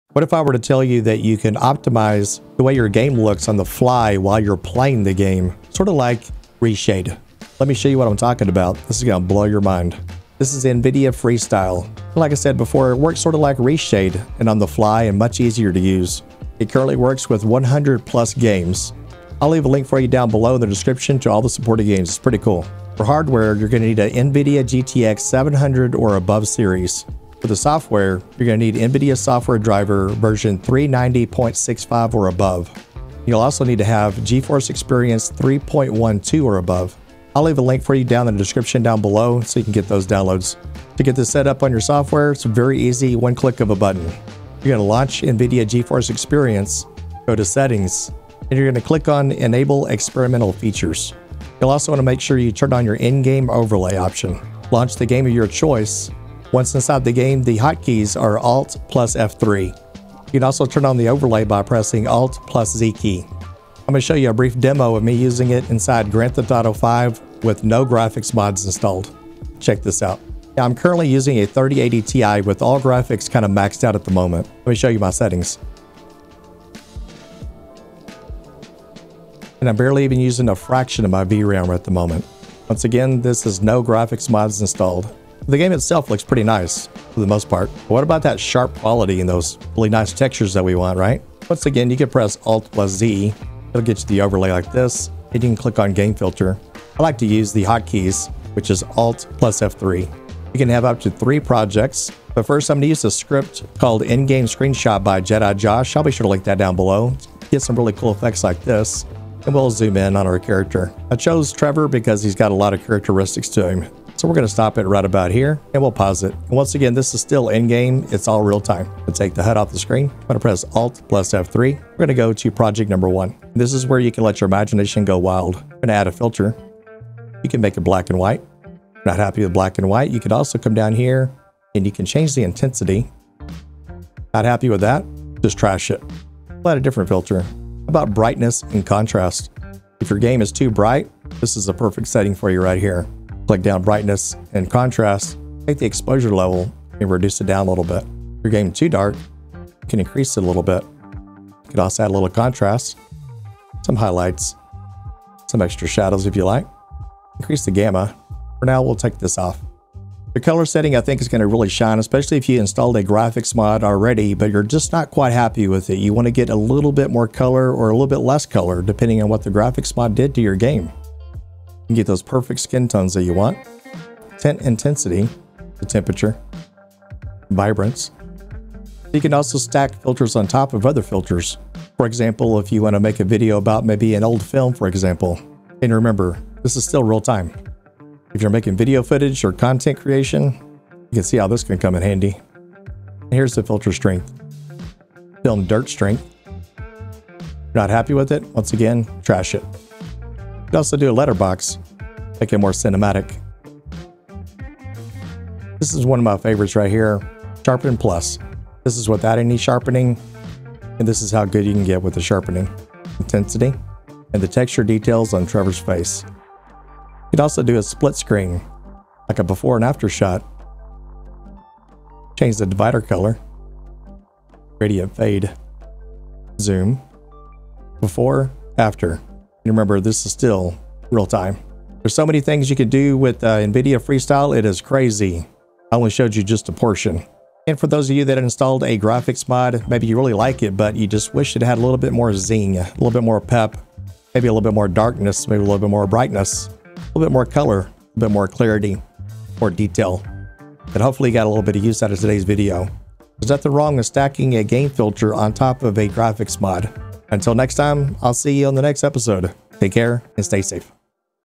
NVIDIA Freestyle - Hidden NVIDIA Feature to Increase Graphics in Real-time - No More Reshade?
Want amazing graphics for FREE? Nvidia Freestyle lets you transform your games in real-time, ditching the hassle of ReShade. Get the perfect look, instantly!

📣Have a YouTube channel?  Are you looking to grow even faster?  Do what I did and look at TubeBuddy